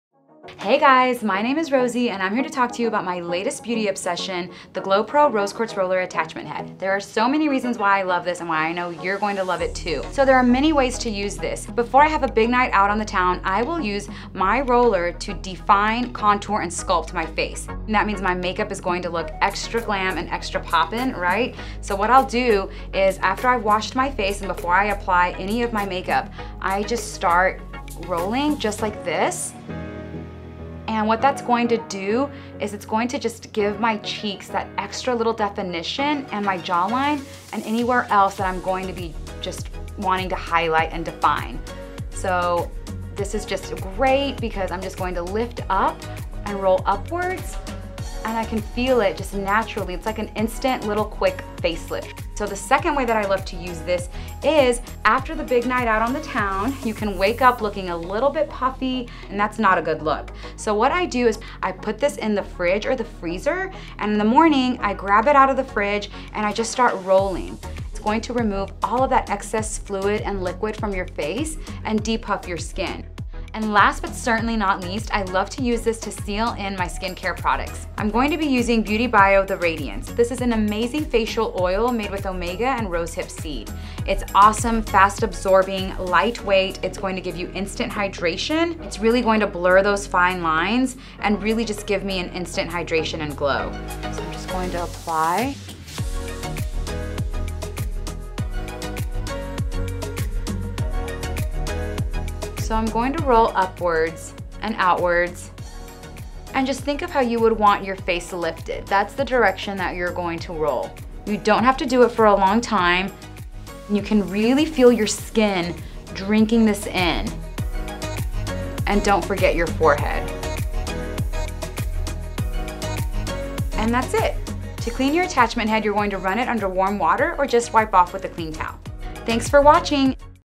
BeautyBio: GloPRO® + Rose Quartz Rolling: How to contour and create flawless, make-up ready skin
How to use BeautyBio’s Rose Quartz GloPRO® attachment to get an instant face lift before applying makeup + how to reduce AM puffiness.